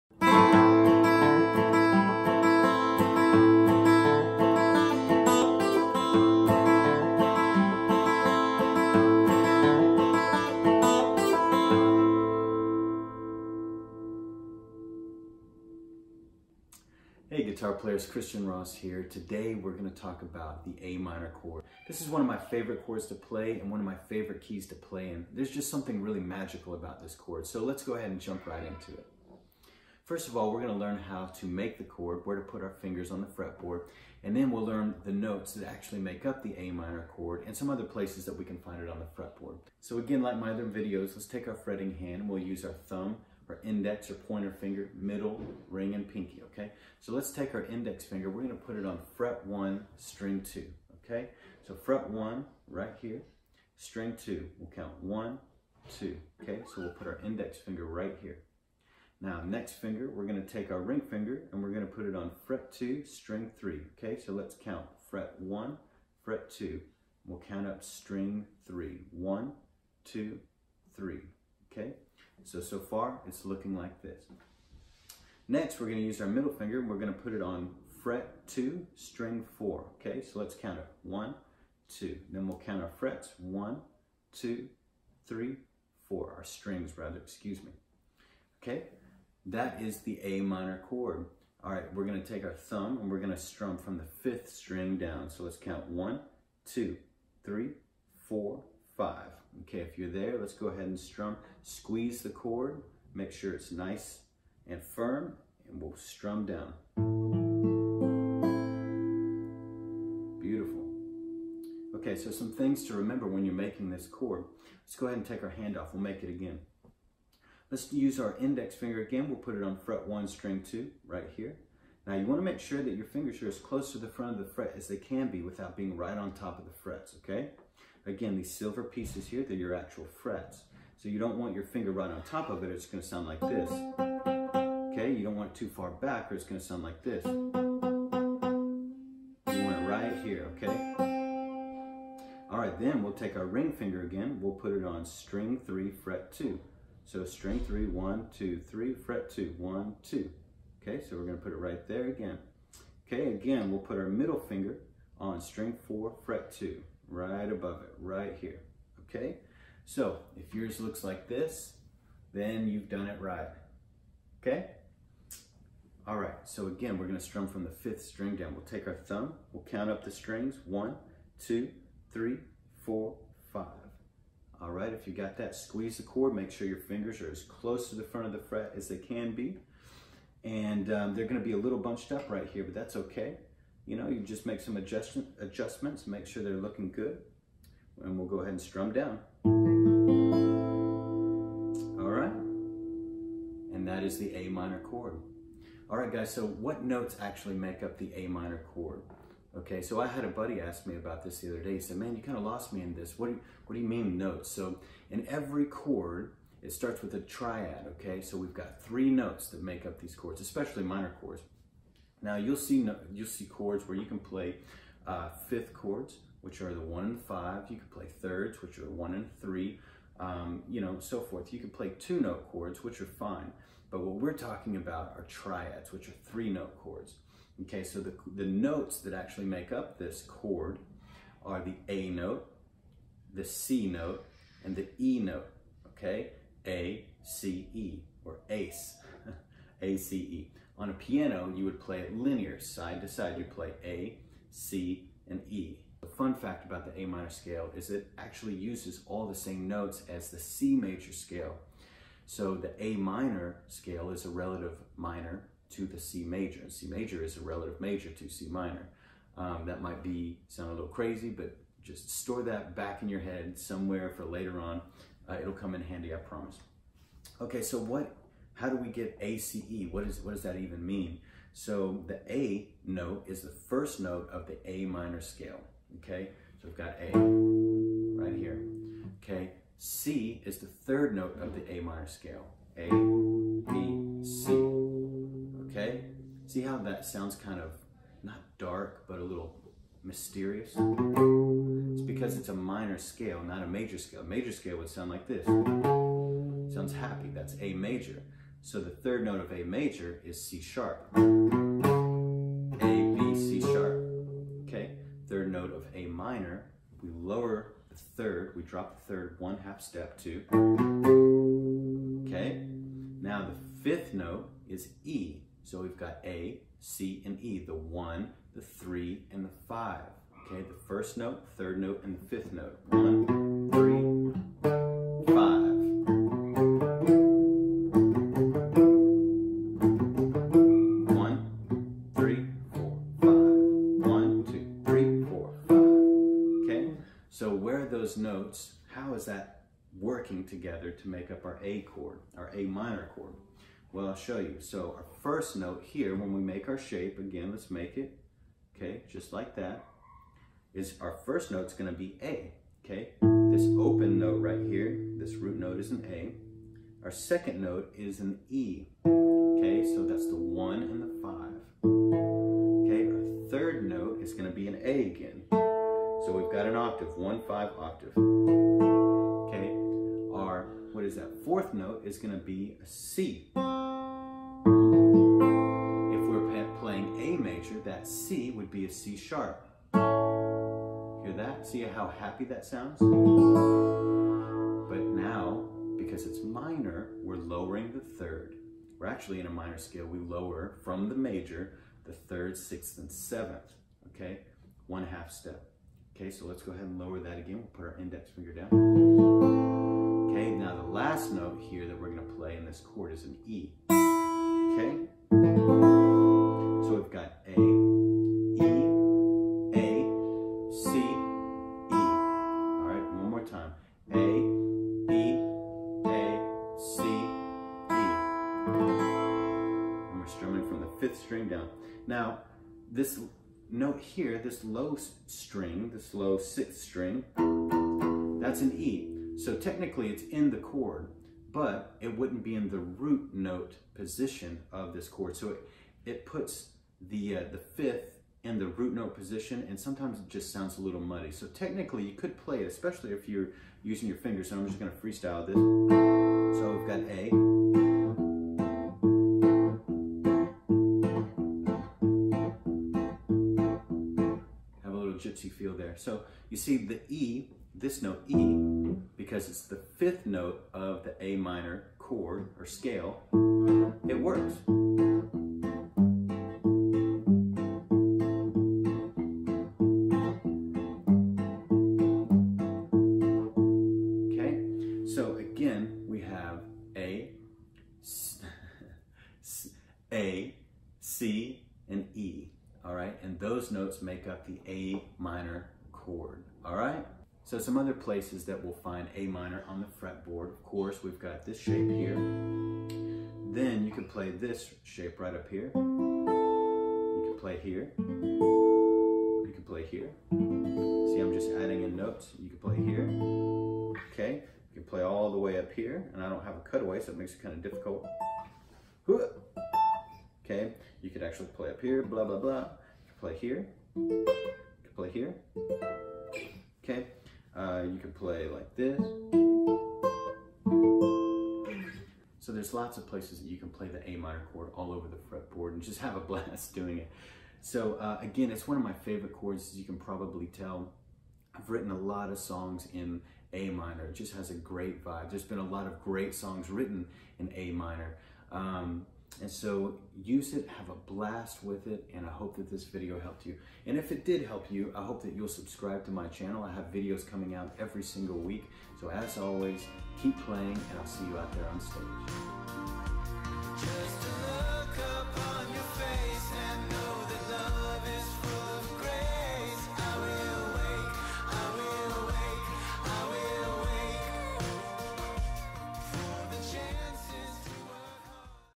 Mastering the A minor (Am) Open Chord - First Beginner Guitar Lessons
Welcome to Mastering the A minor Open Chord!

In this lesson we're talking all about the A minor (Am) open chord. I mean, there's nothing left to say about it...it's all here! More info on the A minor open chord than you could ever hope for...or need. ;) BUT, you'll know exactly what you're doing, how to do it and why. Soooo, let's get to it guitar PLAYAS!

We start by learning how to make the A minor Chord shape and how all our fingers should look on the fret board. Then we'll be looking at the notes that make up the A minor Triad and other places we find the A minor Chord along the fret board. Get ready to rock! One step closer to mastering guitar.

Much love and blessings. You're awesome, keep playing!

-Christian Ross (Heríam)